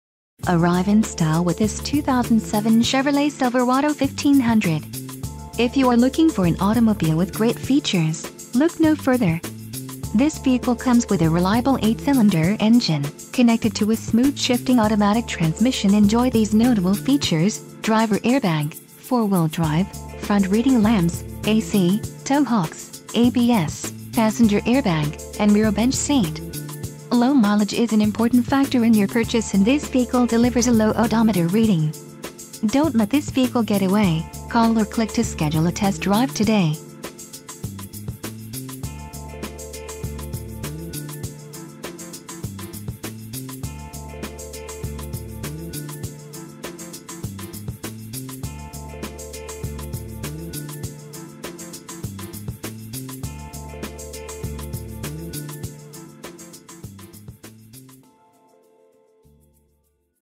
2007 Chevrolet Silverado 1500 - Chapman Chevrolet - Tempe,
Arrive in style with this 2007 Chevrolet Silverado 1500. If you are looking for an automobile with great features, look no further. This vehicle comes with a reliable 8 cylinder engine, connected to a smooth shifting automatic transmission.. Enjoy these notable features: Driver Air Bag, Four Wheel Drive, Front Reading Lamps, A/C, Tow Hooks, ABS, Passenger Air Bag, and Rear Bench Seat. Low mileage is an important factor in your purchase and this vehicle delivers a low odometer reading. .